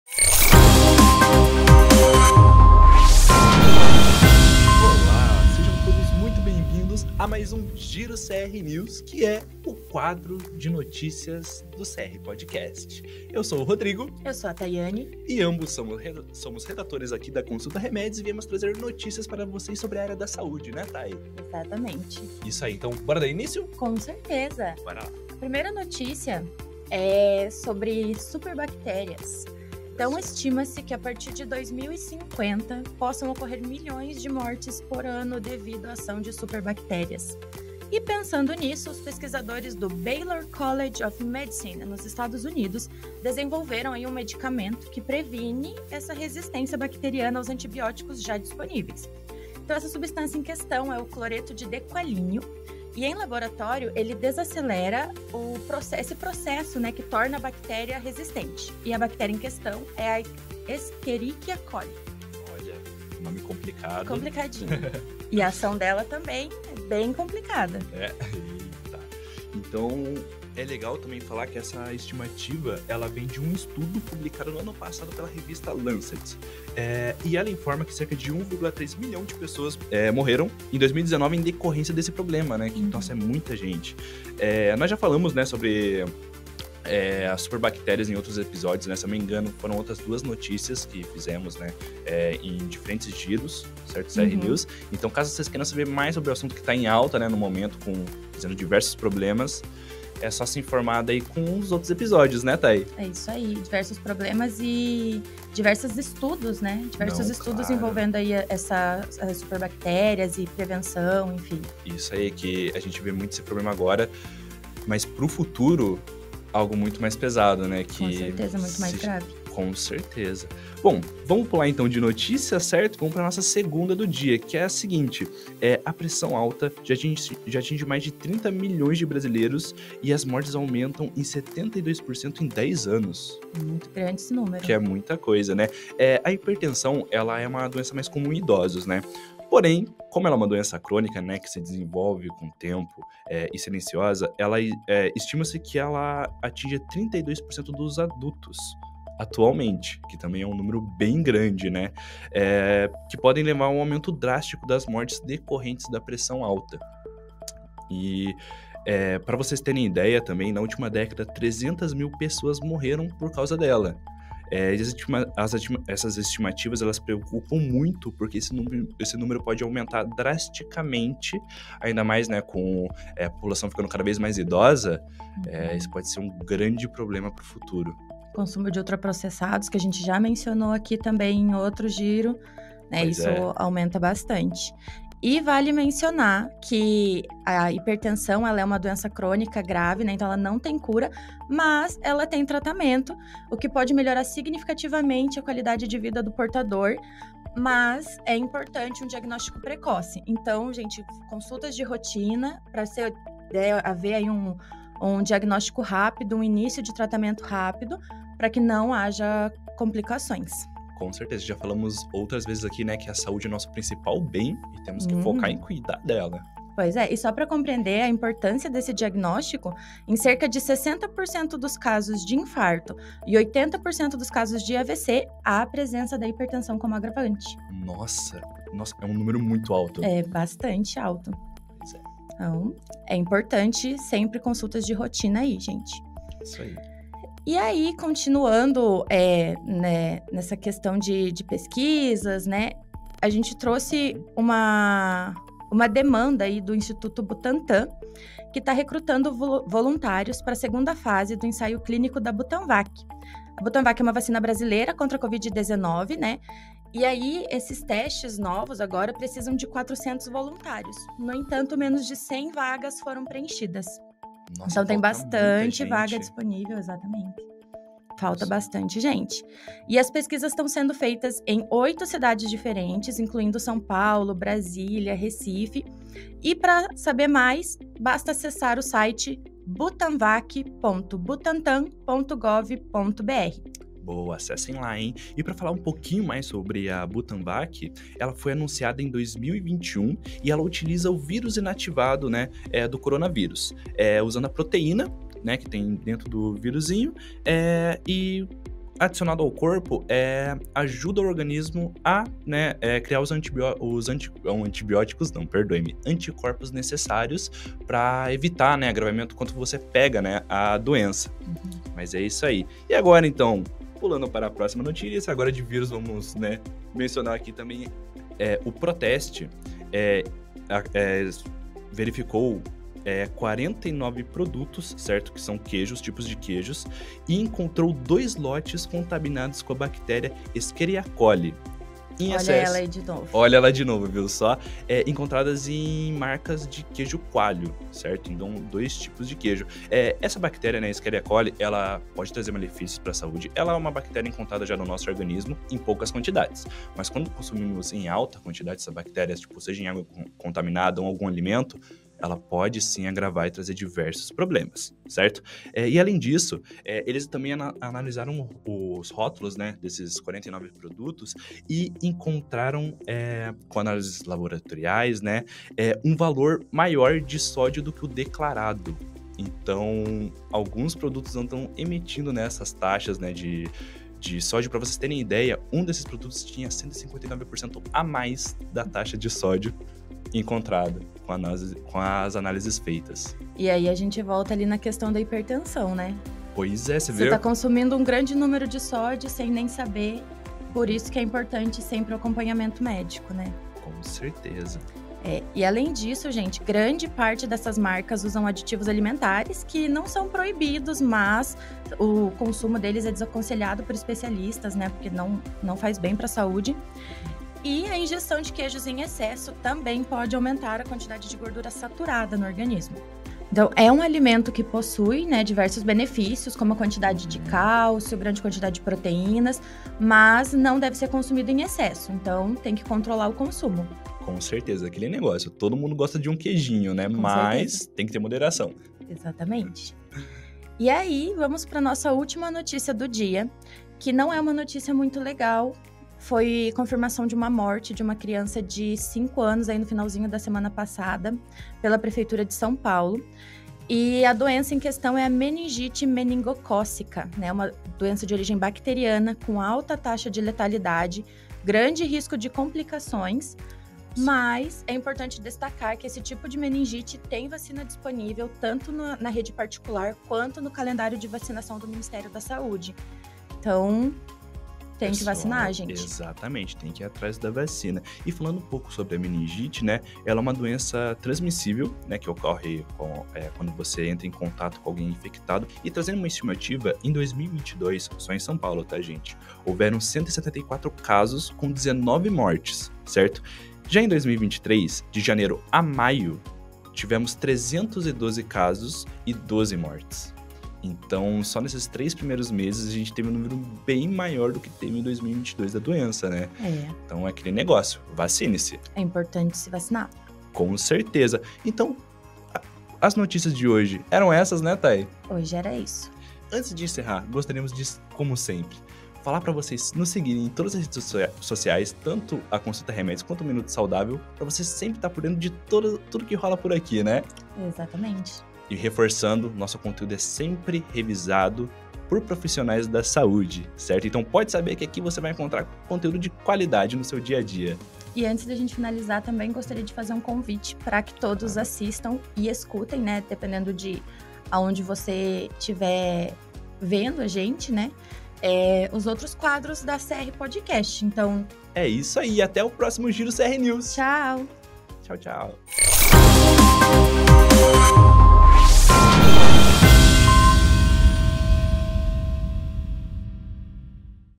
#4 Giro CRnews - Pressão alta, testes da ButanVac, altos casos de meningite e mais | CR Podcast
No Giro CRnews de hoje Taiane Piacecki e Rodrigo Ferens comentam sobre assuntos, como: o alto número de casos de pressão alta; um possível medicamento contra as superbactérias; Proteste encontra bactérias e excesso de sódio em queijos de diferentes marcas e mais.

Acompanhe este episódio e fique por dentro do que está rolando na área da saúde!

Quer saber tudo sobre saúde, doenças, remédios, gravidez e dietas fitness?